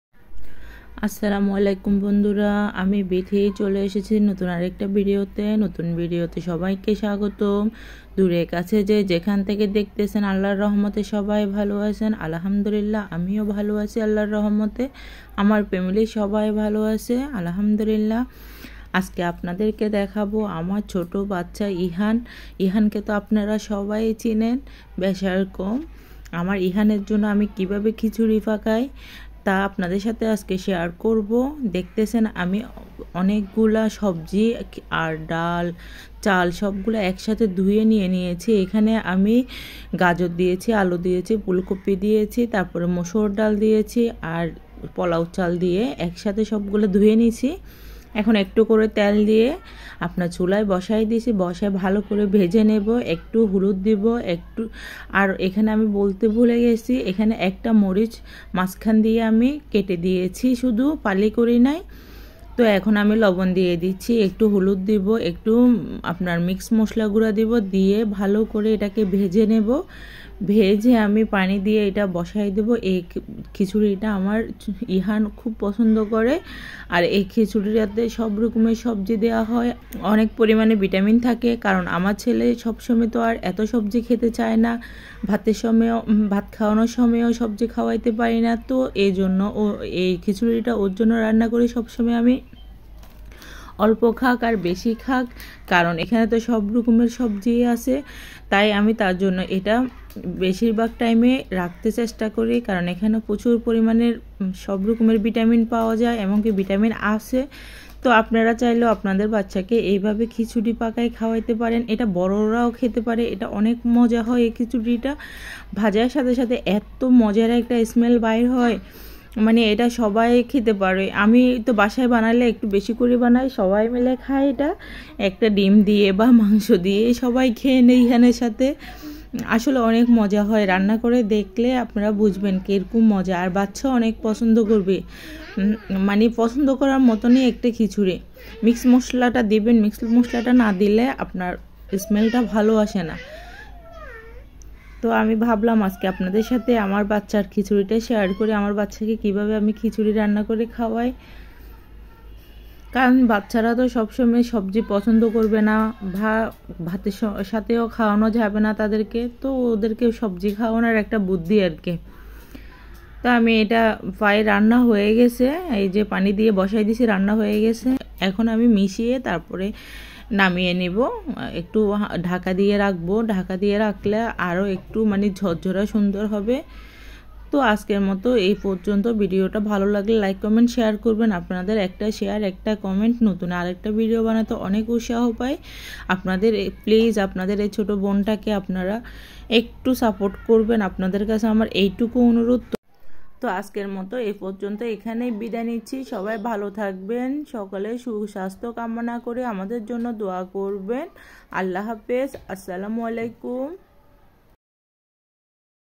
দুই বছরের বাচ্চার জন্য সবজি খিচুড়ি রান্না করলাম || Healthy baby food vegetable khichdi😊
দুই বছরের বাচ্চার জন্য সবজি খিচুড়ি রান্না করলাম।

How to cook vegetable khichuri for two year old baby.

Healthy baby food vegetable khichdi😊